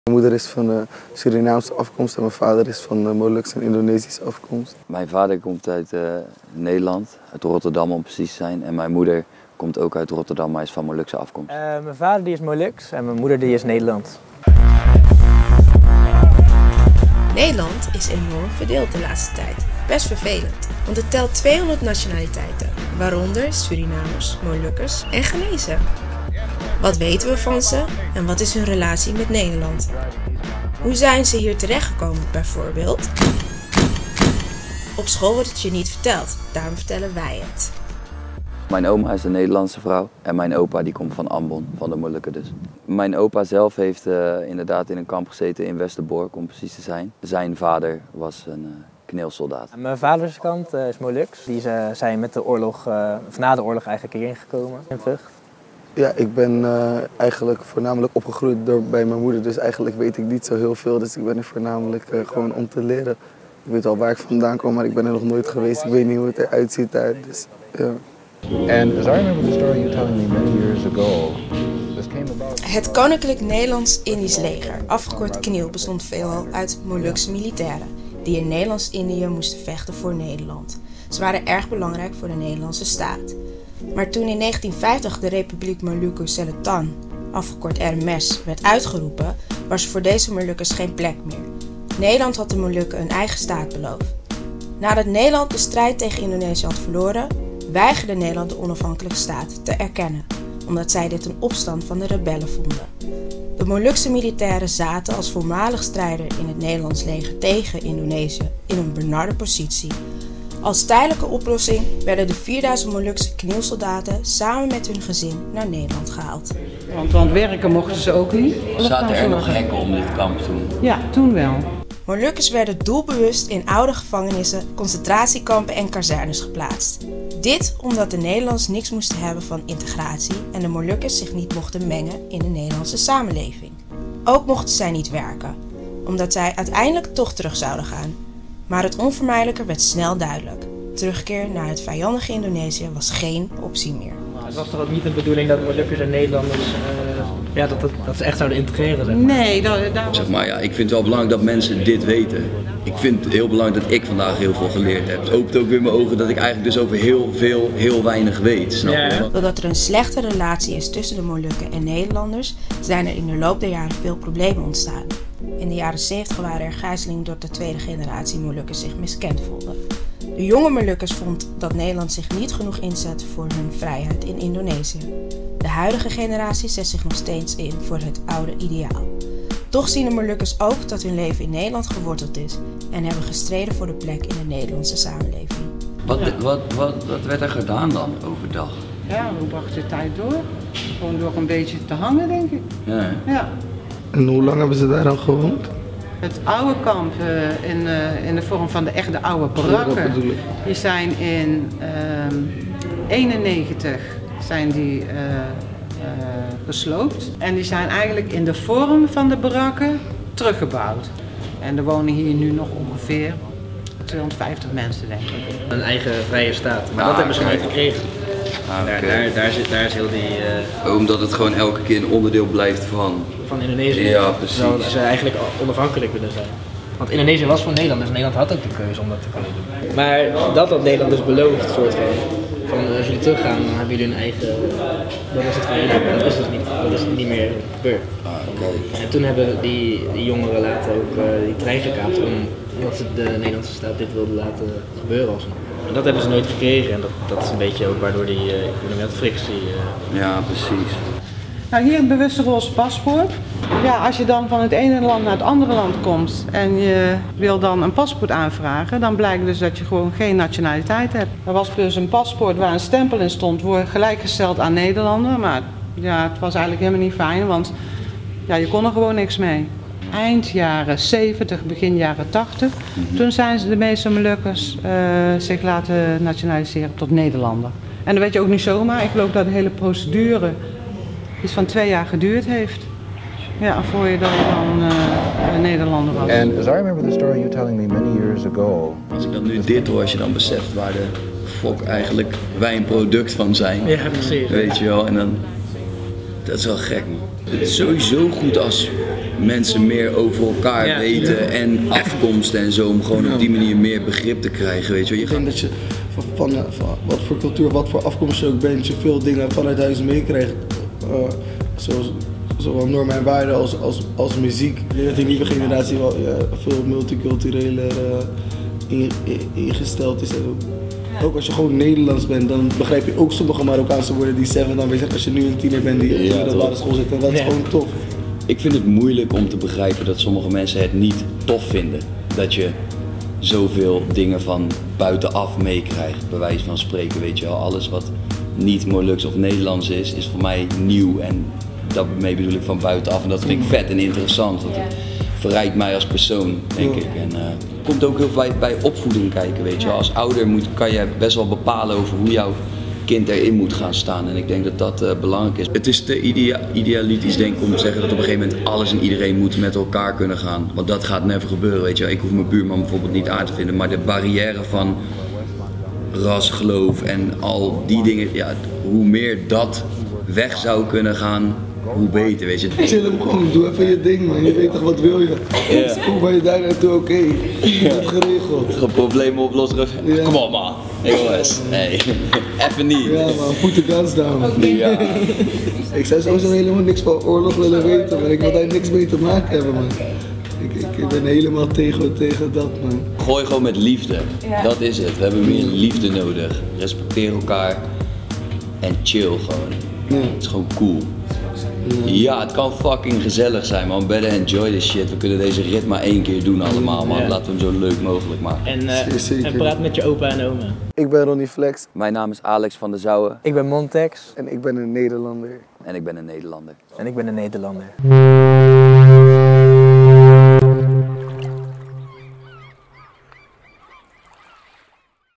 Ik ben een Nederlander - Deel 1 [ROOFSTAAT - 14 maart]
Nederland telt meer dan 200 nationaliteiten, waaronder Molukkers, Surinamers en Ghanezen. Maar wat is hun relatie met Nederland en wat weten de artiesten van hun afkomst? Op school wordt het je niet verteld en daarom vertellen wij het.

ROOFSTAAT door Ewald Vanvugt, 14 maart 2016.

Op maandag 14 maart wordt het geschiedenisboek Roofstaat gepresenteerd. De presentatie is onderdeel van een avondvullend programma in de Paradiso, waar muzikanten, schrijvers en andere sprekers op eigen en persoonlijke wijze ingaan op de schaduwzijde van het Nederlandse koloniale verleden. Met optredens van o.a.: Typhoon, Glen Faria (MC Fit), Fresku,  Karin Amatmoekrim, Kees de Koning en Antoine de Kom.

M.m.v. Nationaal Monument Kamp Vught